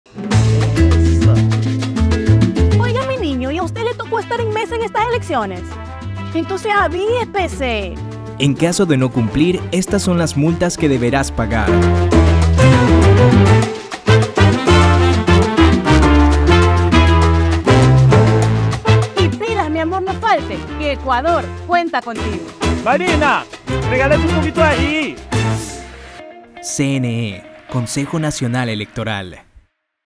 Si faltas, hay multa. ¡Ponte pilas! Ecuador cuenta contigo - Elecciones anticipadas 2023
¿Eres parte de una Junta Receptora del Voto?🗣️🗳️

¡No faltes a conformar la mesa! Tu participación asegura la transparencia y fortalece la democracia en estas EleccionesAnticipadas2023Ec.

🗳️💪 Si no asistes, deberás cancelar una multa. Ponte pilas y evita sanciones.

Asiste el domingo 20 de agosto y forma parte de esta fiesta  democrática.